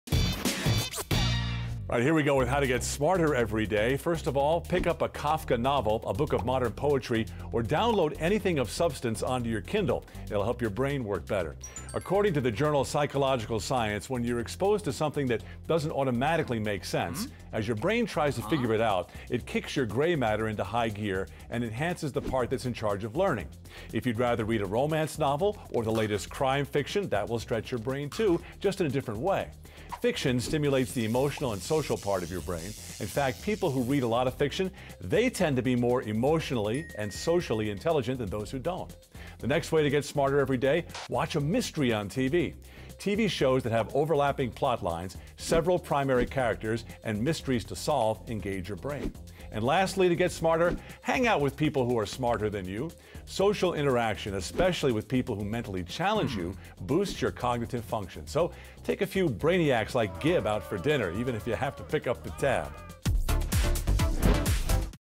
How to be SMARTER - Habits That Will Make You Smarter
First of all, pick up a Kafka novel, a book of modern poetry, or download anything of substance onto your kindle. It'll help your brain work better. According to the journal psychological science, when you're exposed to something that doesn't automatically make sense, as your brain tries to figure it out, it kicks your gray matter into high gear. It enhances the part in charge of learning.

If you'd instead read a romance novel or the latest crime fiction, that will stretch your brain, too, just differently. Fiction stimulates the emotional and social parts of your brain. In fact, people who read a lot of fiction tend to be more emotionally and socially intelligent than those who don't.

The following way is to watch a mystery on tv. Tv shows with overlapping plot lines, several primary characters, and mysteries to solve engage your brain.

And lastly, hang out with people who are more intelligent than you. Social interaction, especially with mentally challenging people, boosts your cognitive function.

wanna be smart?